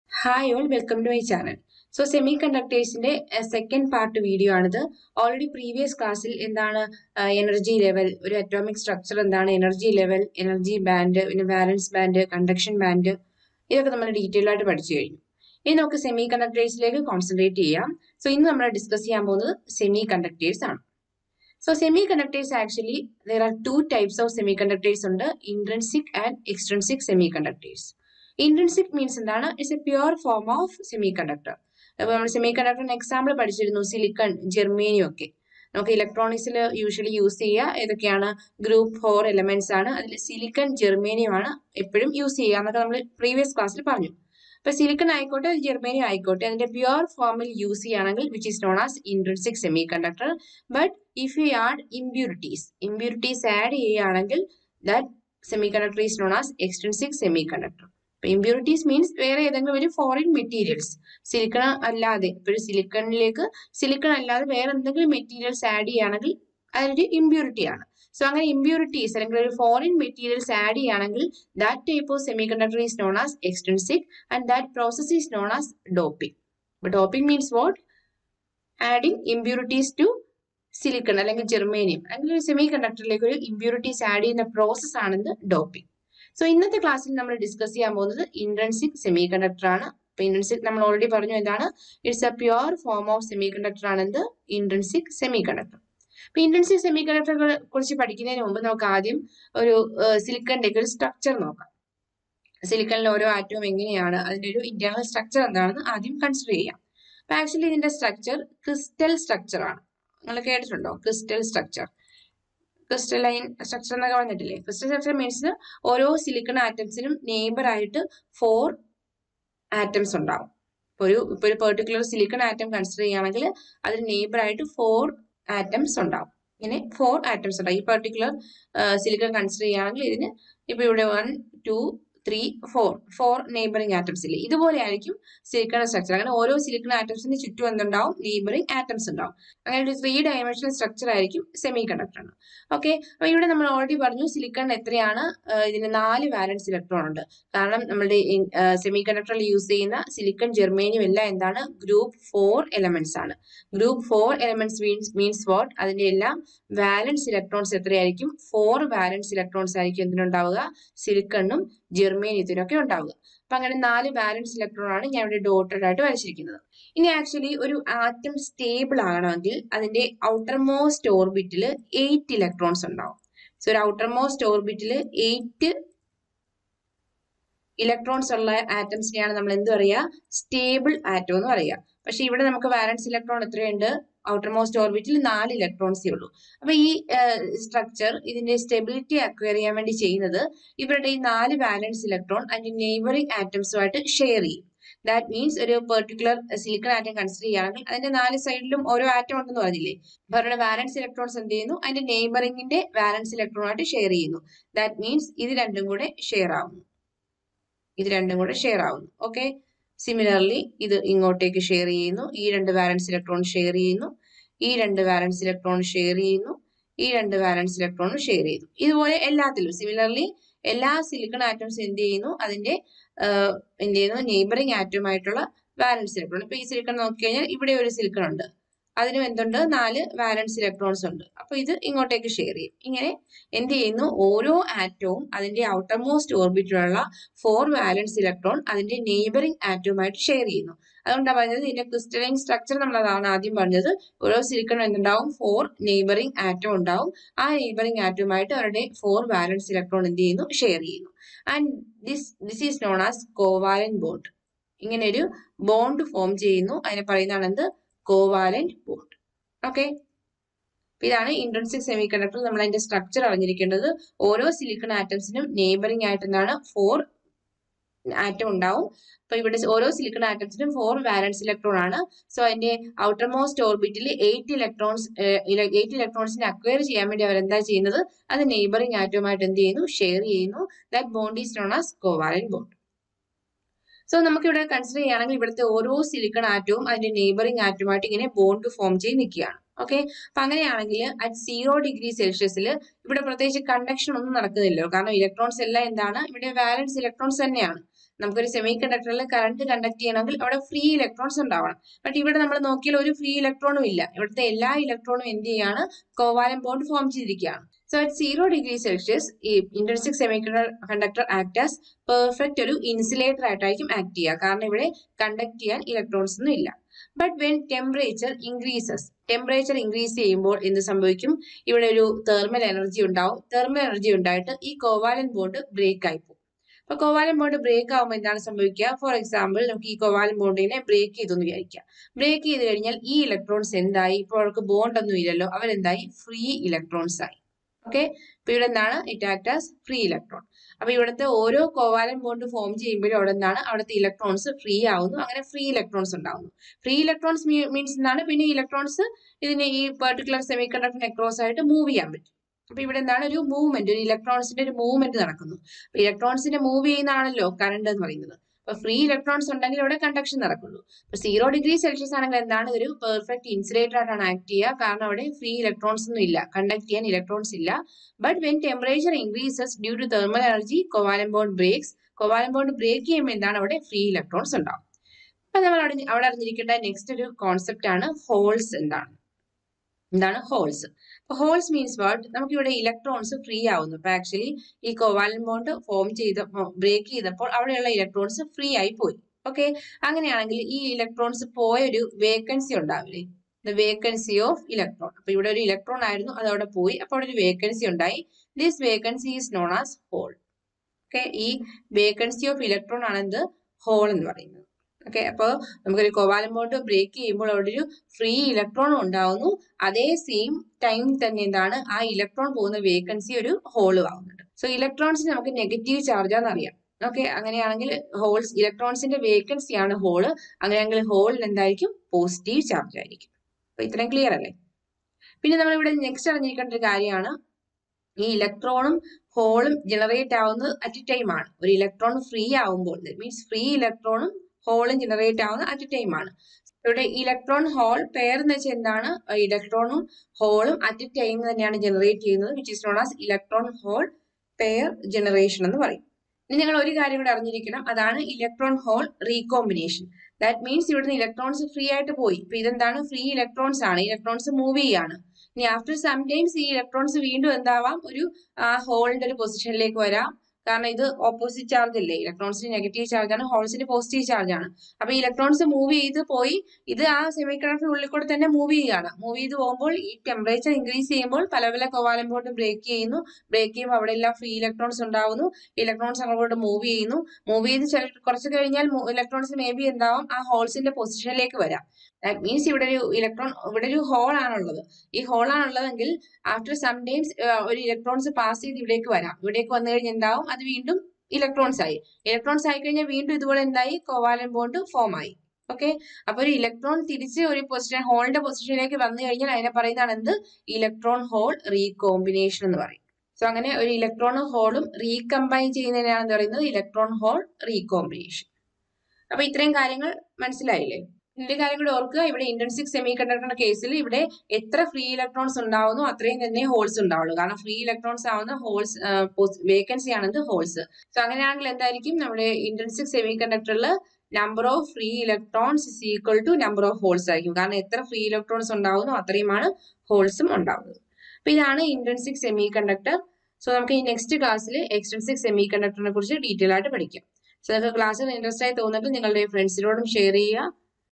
Intrinsic semiconductor| electron-hole pair| electron hole recombination| Malayalam explanation
This video explains about intrinsic semiconductor.